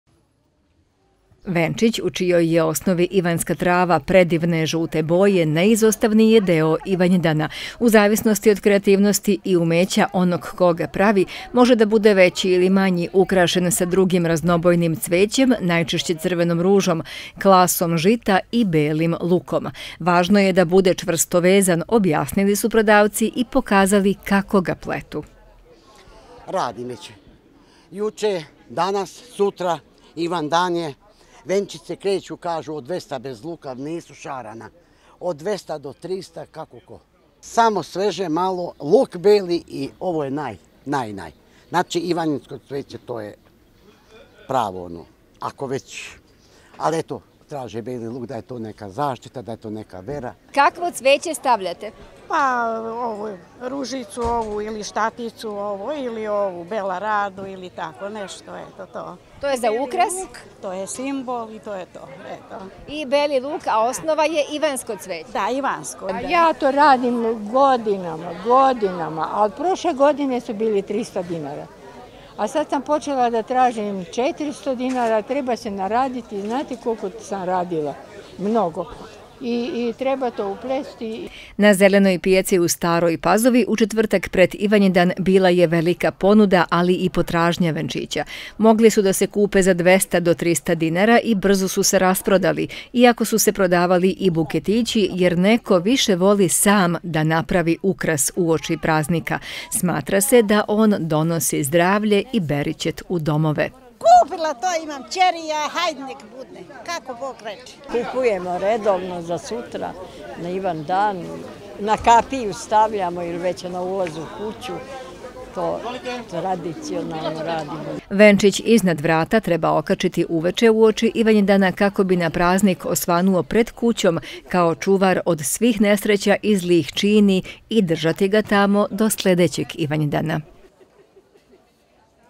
Ivanjski venac na svaku kapiju
Uoči Svetog Jovana Preteče, u narodu poznatijeg kao Ivanjdan, žene širom Srbije pletu venčiće od cveća i uveče ga stavljaju ispred kućnih vrata. Ako venac od ovih naročitih cvetova na praznik osvane pred kućom, veruje se da će porodicu zaštititi od svake nedaće. To su  narodna verovanja o kojima smo razgovarali sa sugrađanima na staropazovačkoj zelenoj pijaci, gde je bila velika ponuda venčića i buketića od ivanjskog cveća.